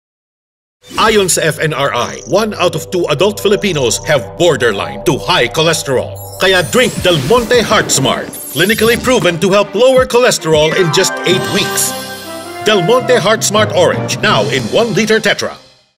Manage your cholesterol, drink Del Monte Heart Smart!
According to FNRI, 1 out of 2 Filipino adults today have borderline to high cholesterol.
You can help manage your cholesterol by drinking Del Monte Heart Smart.
Watch the video and learn how this deliciously refreshing drink can help you fight cholesterol.